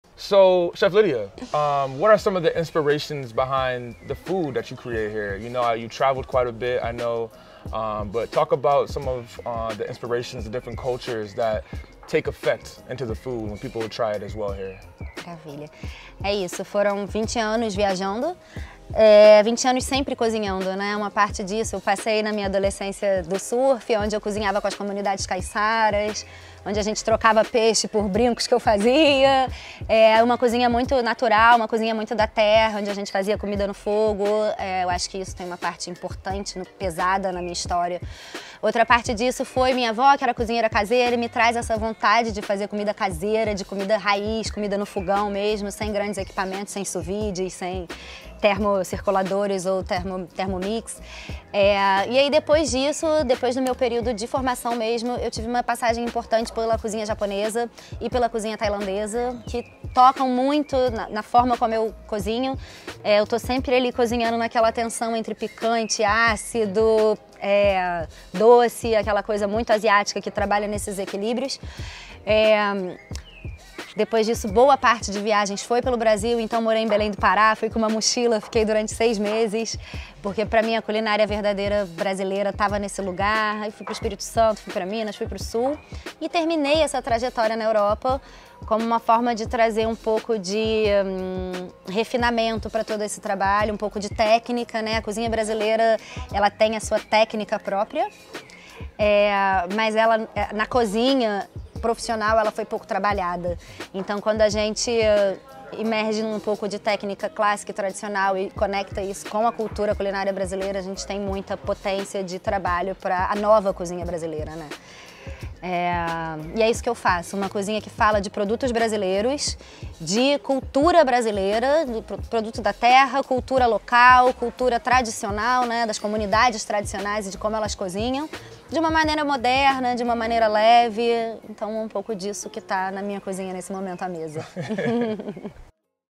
FINDING INSPIRATION TO CREATE YOUR DREAM | CHEF LYDIA GONZALEZ
What does it mean to create your dream? Whether you're inspired to become a chef or in a completely different industry - dare to dream! Chef Lydia Gonzalez breaks down how she was able to make her dream of opening up her own restaurant in her city of Petropolis come true.

Tune in!

In this amazing episode, we caught up with Chef Lydia Gonzalez who runs ANGA ATELIE CULINARIO in Petropolis, just outside Rio De Janiero. Chef Lydia guides us through her restaurant which basks in a nature environment. During this hour trip up to petrópolis, Chef Lydia runs through her experience as a chef and why she wanted to start her own restaurant in the very place she grew up after traveling for years sharpening or sword as a chef. Strap your seatbelt in this truly OFF THE MENU episode and get ready to take a trip to Rio De Janeiro. Enjoy!